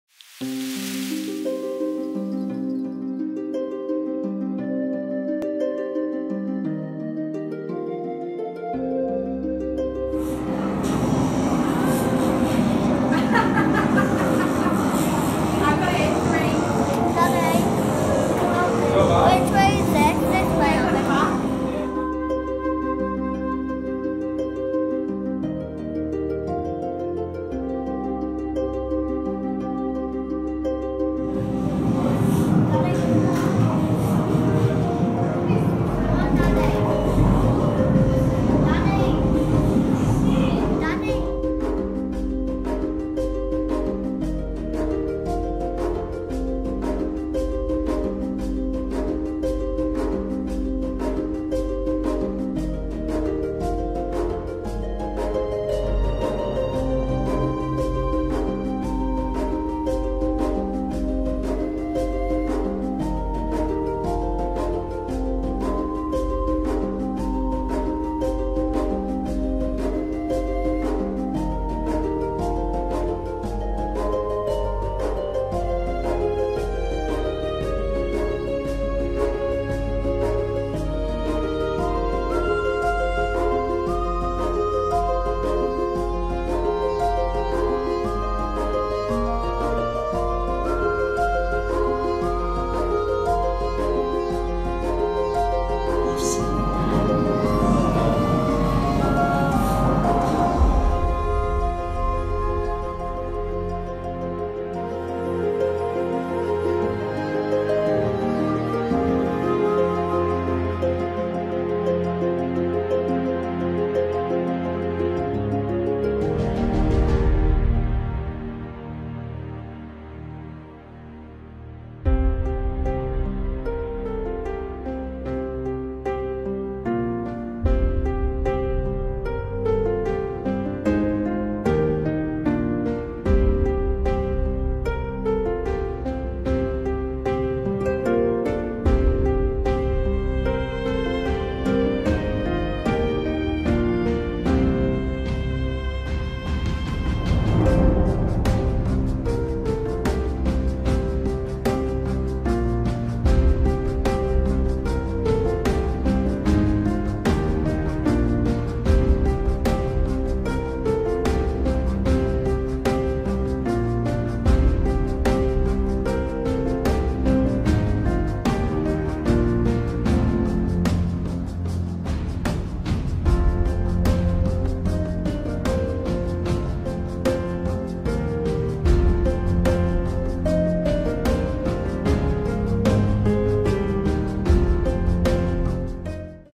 Atlantis
Legend has it that the sea god Poseidon himself protects the ancient underwater city. Explore the depths of the Ocean and encounter 'The Kraken', sharks and ancient shipwrecks. Finally, face the fury of Poseidon inside the lost city of Atlantis.